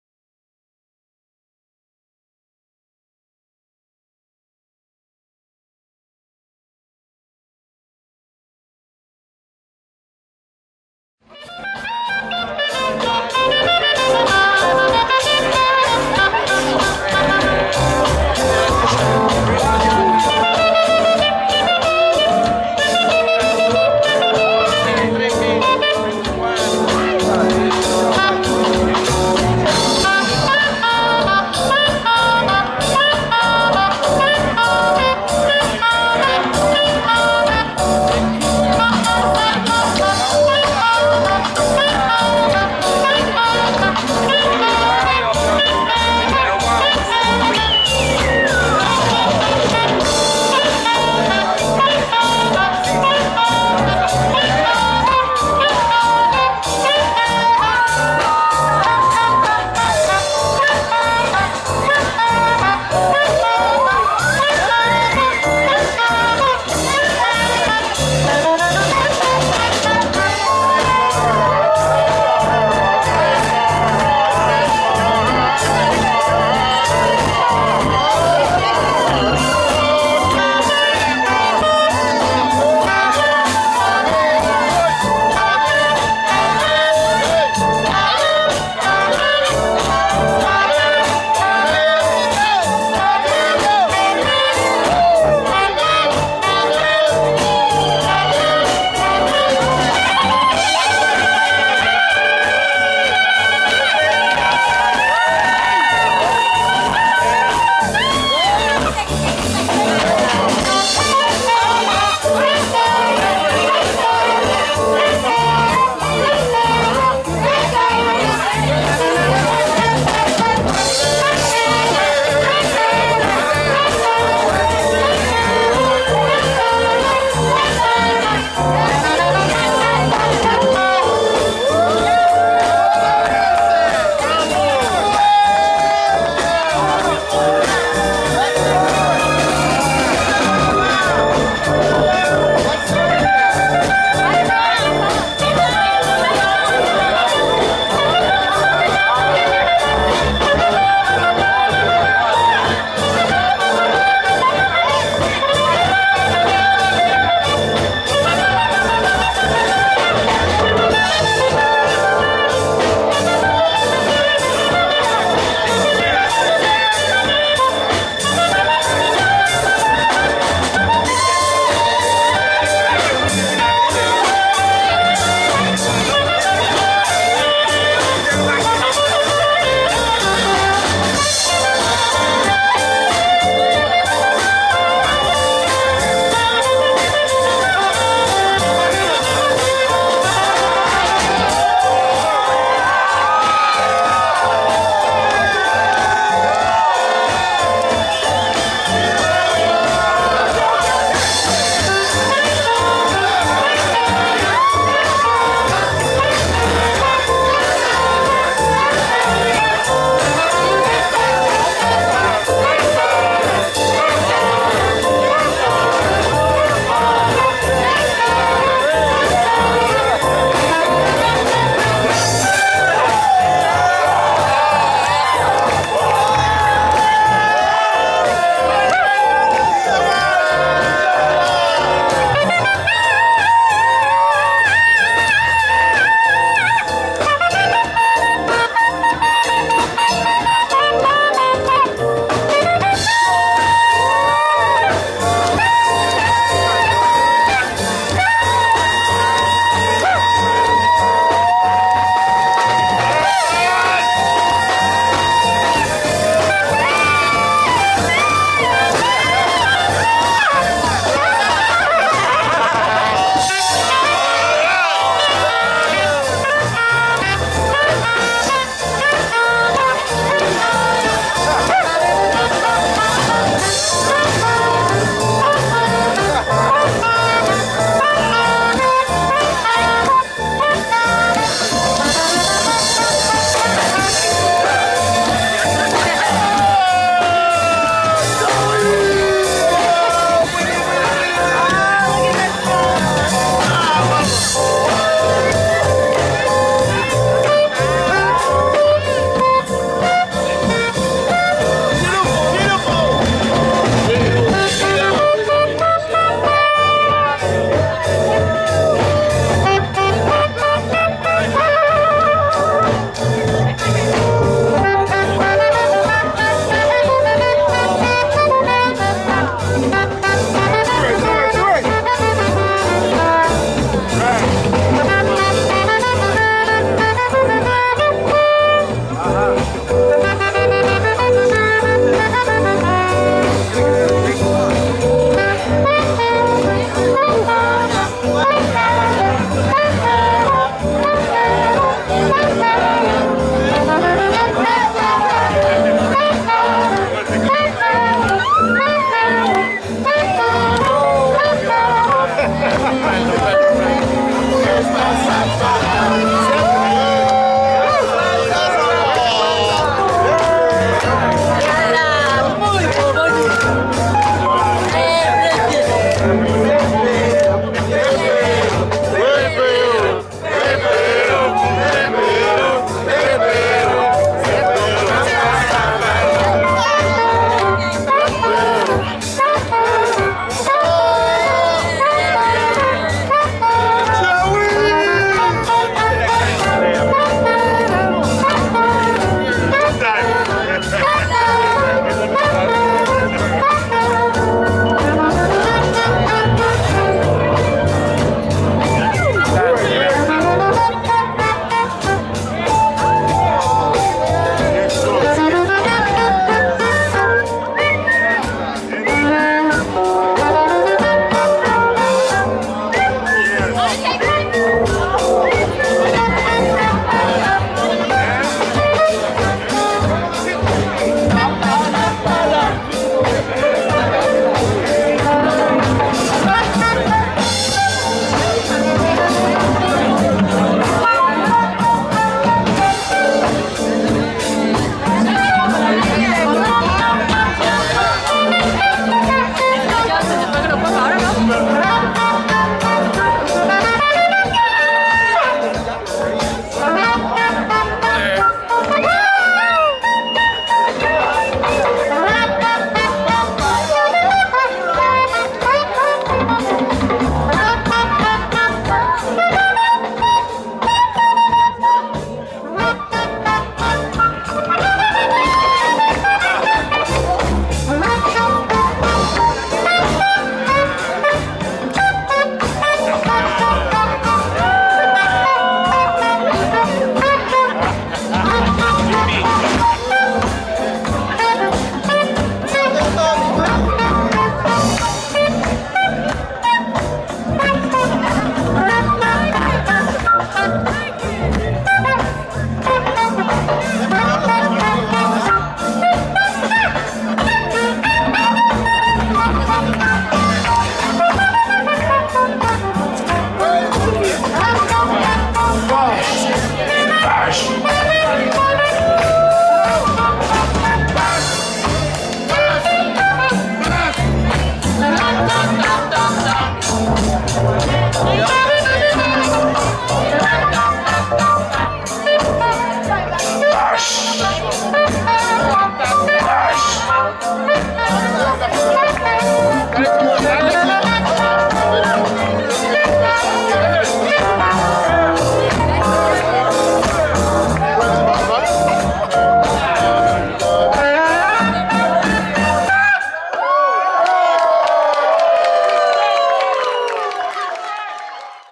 Jowee Omicil Miami Little Haiti Jan. 20th 2012 part 2
Big night in little Haiti with Jowee Omicil January 20th 2012